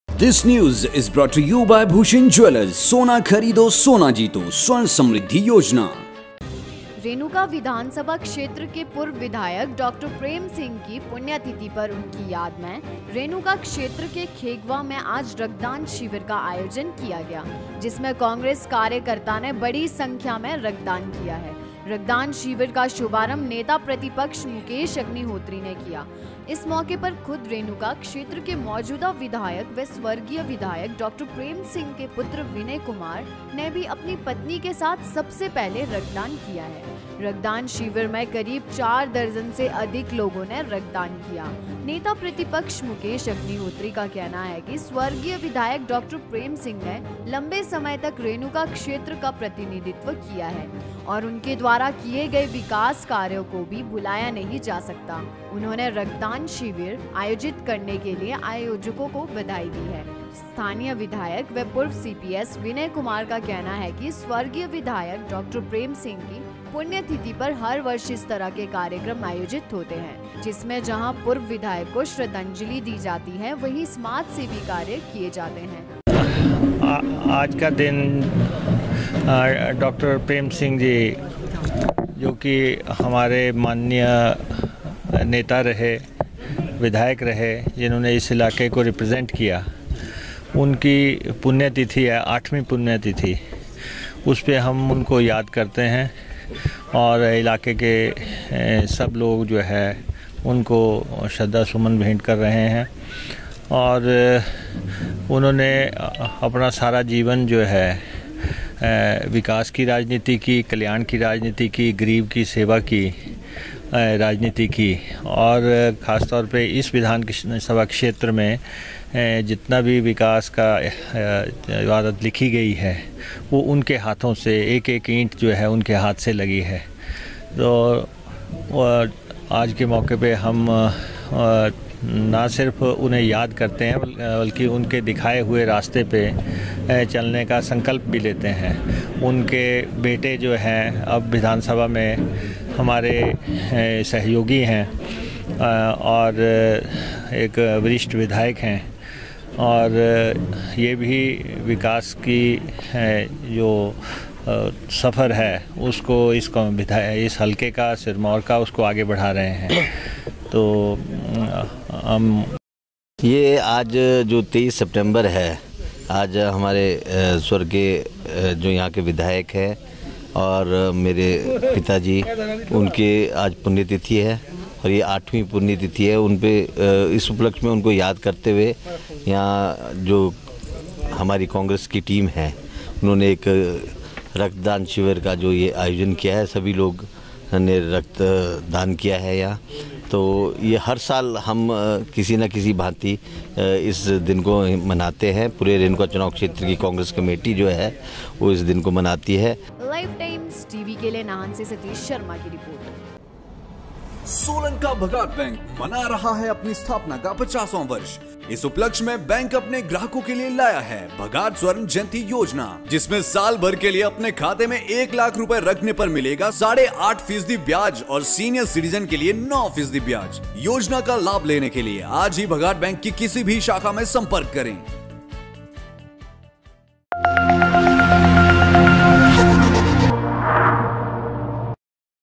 नाहन : पूर्व विधायक की पुण्यतिथि पर रक्तदान शिविर का आयोजन ।
नाहन : पूर्व विधायक की पुण्यतिथि पर रक्तदान शिविर का आयोजन ,कांग्रेस कार्यकर्ता ने बड़ी संख्या में किया रक्तदान ।